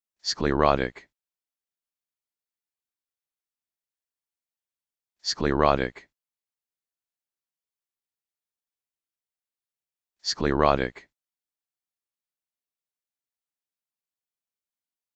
How to Pronounce Sclerotic
Sclerotic - your guide to pronouncing this word, brought to you for free by uPronounce.